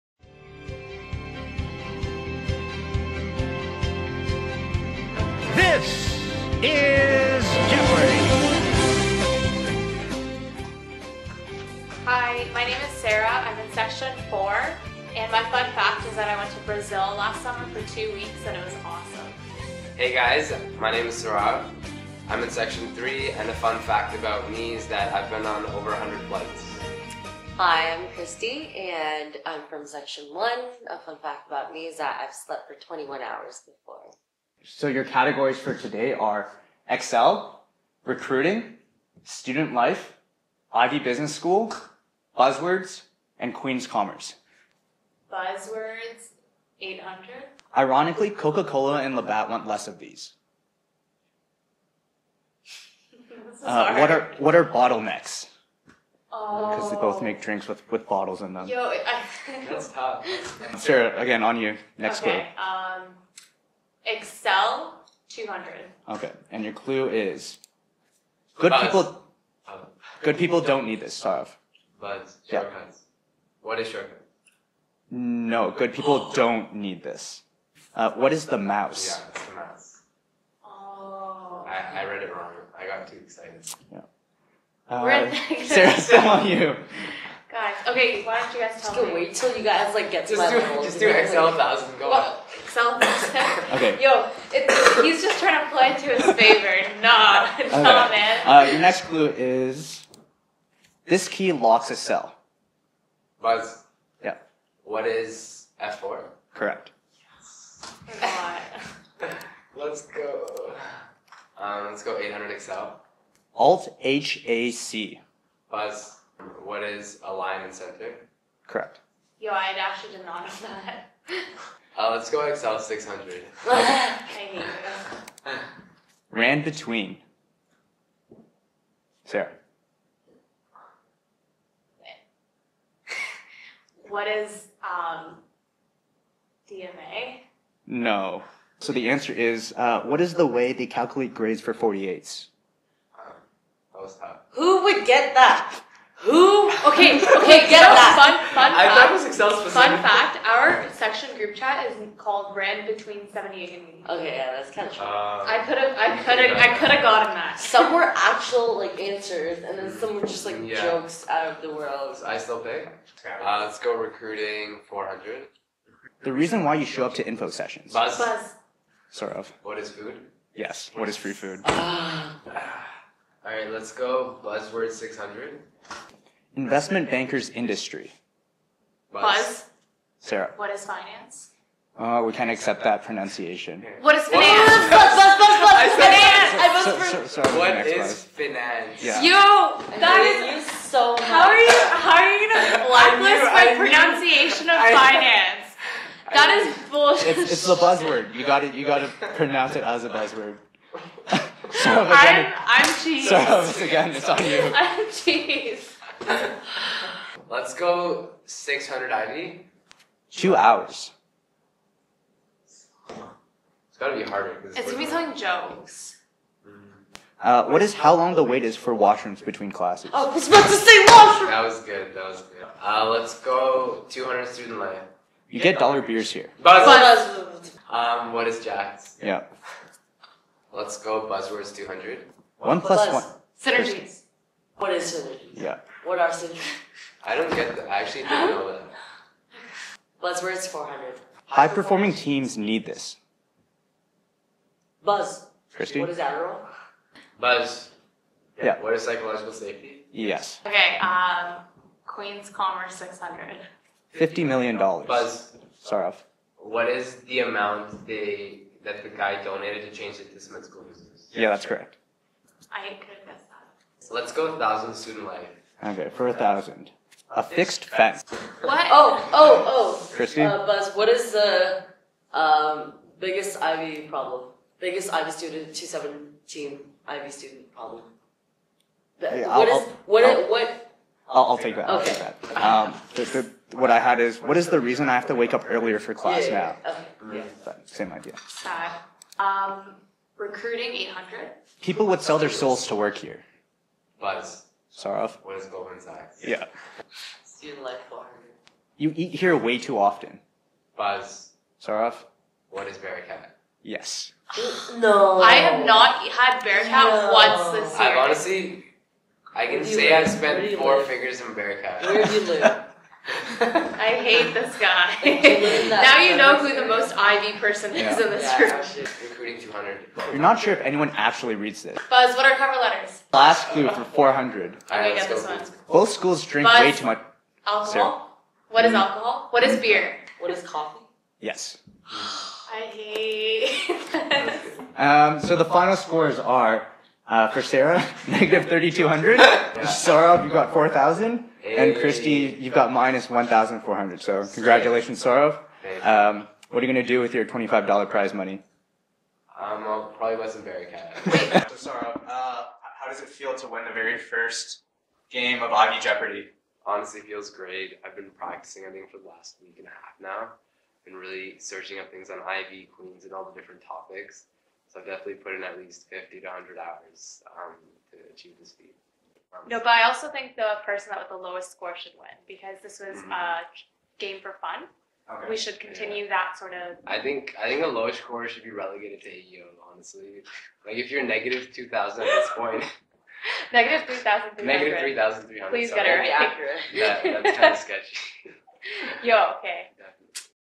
Ivey Jeopardy (HBAA TV: January 2018)
HBA students learn how to tackle real world cases at Ivey, but can they handle them when they're in a game of Jeopardy?

CREATED BY: Nathan Li

FEATURING:
- Saurav Bubber
- Kristy Liu
- Sara Raza